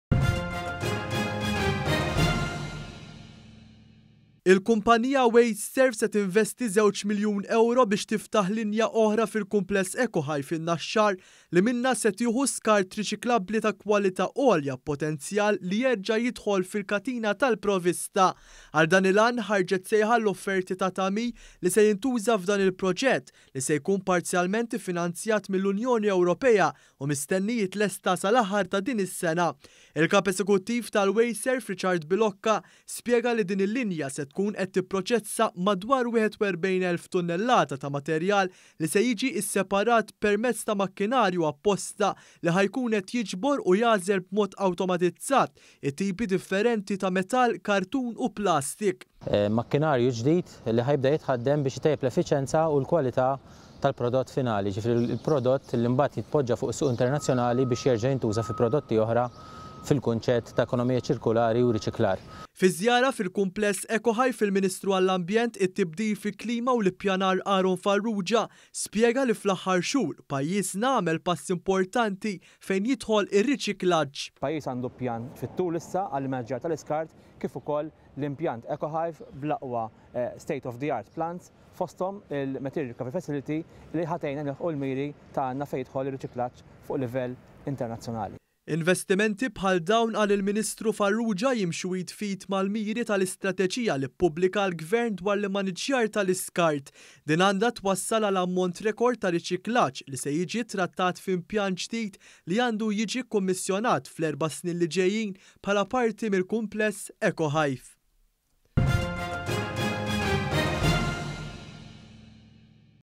INVESTIMENT FAVUR AKTAR SEPARAZZJONI TAL-ISKART
ONE.COM.MT • Il-kumpanija Wasteserv se tinvesti maż-żewġ miljun ewro biex tiftaħ linja oħra ta’ separazzjoni tal-iskart fil-kumpless ECOHIVE fin-Naxxar, li minnha se tkun qed tiġbor u tagħżel b’mod awtomatiku tipi differenti ta’skart b’potenzjal li jerġa’ jidħol fil-katina tal-provista.

KITBA – FRANCESCO BORG
FILMATI – MECP

Il-kumpanija WasteServ se tinvesti żewġ miljun ewro biex tiftaħ linja oħra fil-Kumpless ECOHIVE fin-Naxxar, li minnha se tieħu skart riċiklabbli ta’ kwalità għolja b’potenzjal li jerġa’ jidħol fil-katina tal-provista.

Għal dan il-għan, ħarġet sejħa għall-offerti għat-tagħmir li se jintuża f’dan il-proġett, li se jkun parzjalment iffinanzjat mill-Unjoni Ewropea u mistenni jitlesta sal-aħħar ta’ din is-sena.
Il-Kap Eżeuttiv tal-Wasteserv Richard Bilocca spjega li din il-linja se tkun qed tipproċessa madwar wieħed u erbgħin elf tunnellata ta’ materjal li se jiġi sseparat permezz ta’ makkinarju apposta li ħa jkun qed jiġbor u jagħżel b’mod awtomatizzat it-tipi differenti ta’ metall, kartun u plastik.

RICHARD BILOCCA
KAP EŻEKUTTIV TAL-WASTESERV
“Makkinarju ġdid li għandu jitħaddem biex itejjeb l-effiċjenza u l-kwalità tal-prodott finali. Jiġifieri, il-prodott li mbagħad jitqiegħed fis-suq internazzjonali għall-użu mill-ġdid fi prodotti oħra fl-ekonomija ċirkolari u riċiklata.”

Fi żjara fil-kumpless ECOHIVE, il-Ministru għall-Ambjent, it-Tibdil fil-Klima u l-Ippjanar Aaron Farrugia spjega li fl-aħħar xhur, pajjiżna għamel passi importatni fejn jidħol ir-riċiklaġġ.

AARON FARRUGIA
MINISTRU
“Il-pajjiż issa għandu pjan fit-tul għall-immaniġġjar tal-iskart kif ukoll l-impjant ECOHIVE, b’impjanti state-of-the-art inkluż material cover facility li se tgħin biex nilħqu l-miri tagħna fejn jidħol riċiklaġġ fuq livell internazzjonali.”

Investimenti bħal dawn, qal il-Ministru Farrugia, jimxu id f’id mal-miri tal-istrateġija li ppubblika l-Gvern dwar l-immaniġġjar tal-iskart.

Din għandha twassal għal ammont rekord ta’ riċiklaġġ li se jiġi trattat f’impjant ġdid li għandu jiiġi kkummisjonat fl-erba’ snin li ġejjin bħala parti mill-kumpless ECOHIVE.